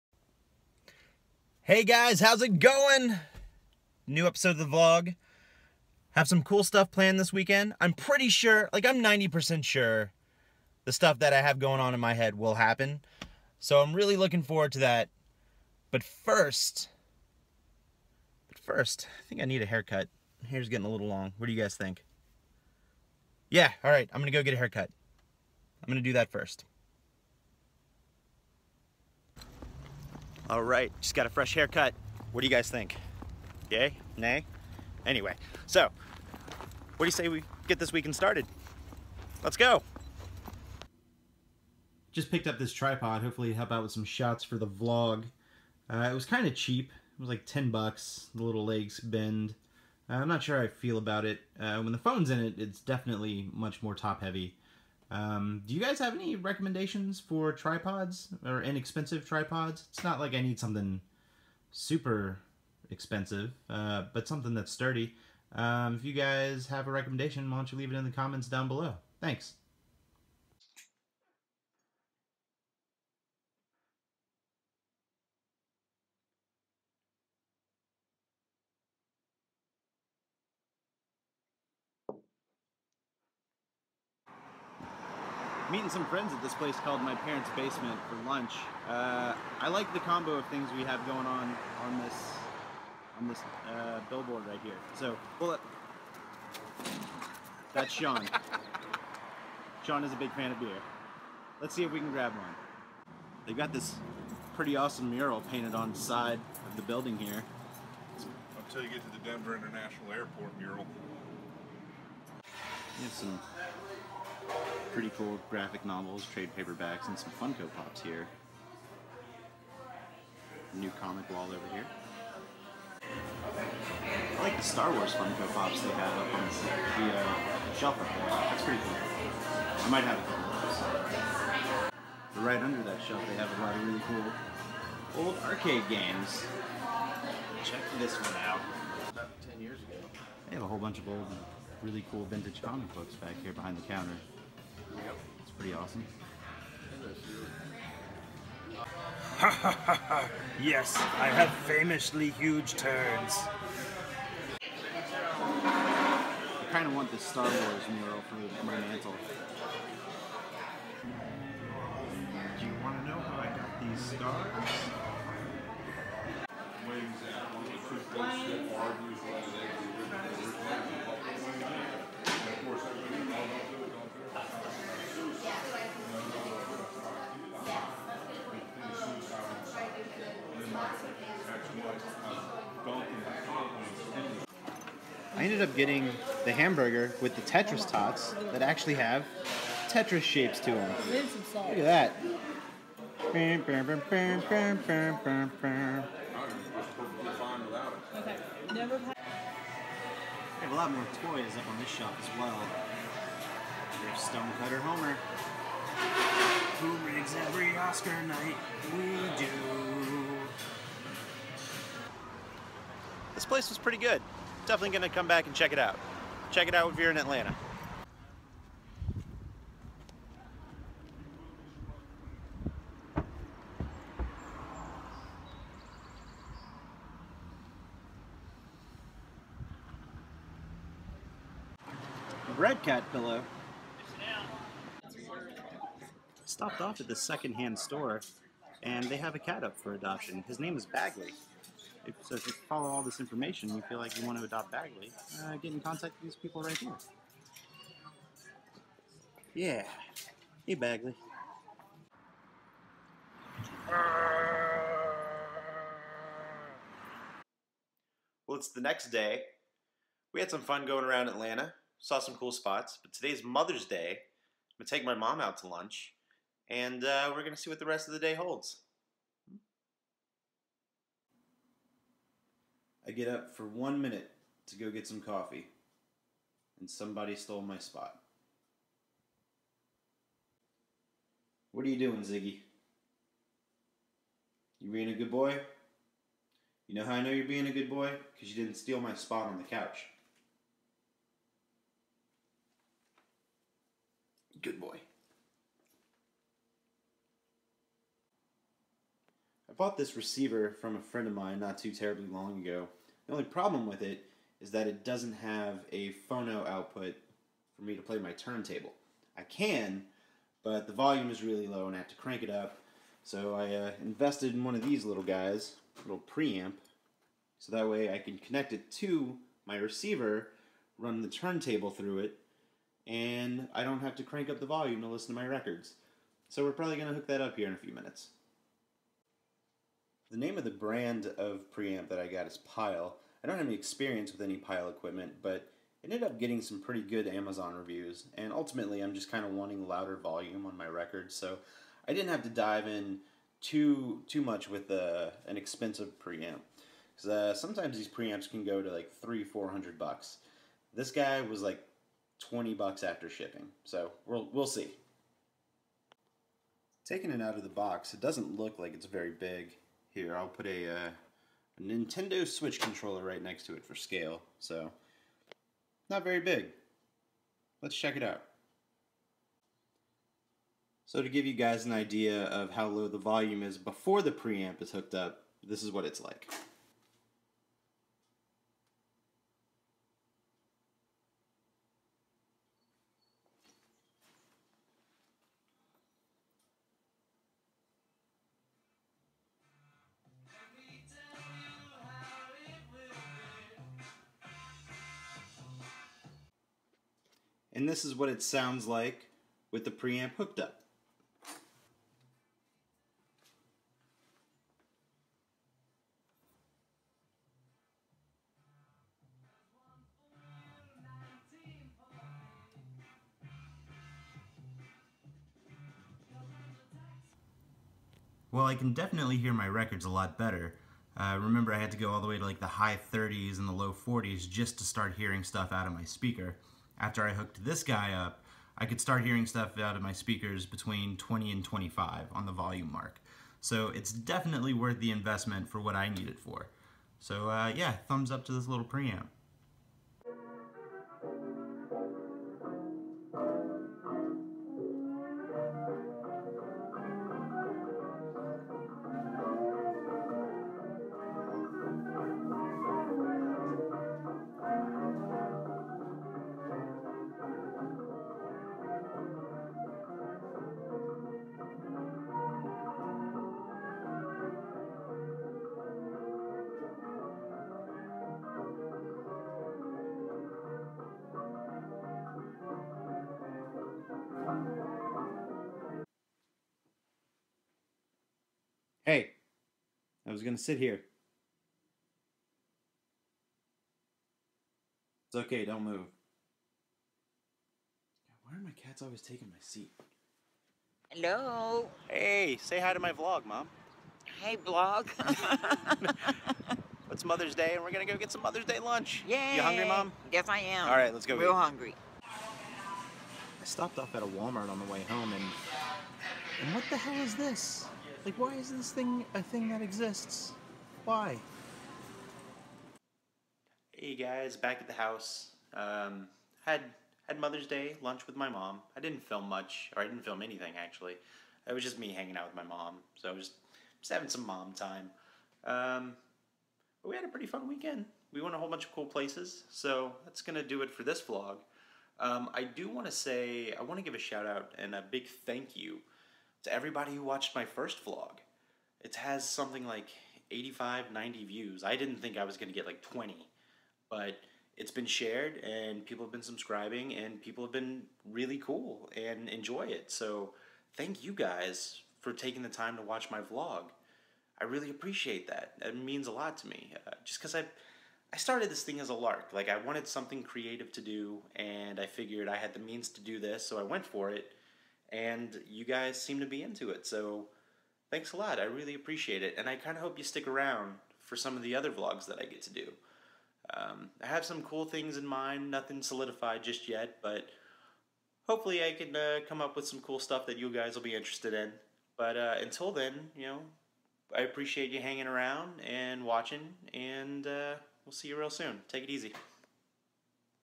Comic Book Store/Bar and My Seat Keeps Getting Stolen
This week we visit a comic book store/bar, I can properly listen to my records again, my horde (horde?) of cats makes their first appearance, and I hang out with my mom for Mother's Day.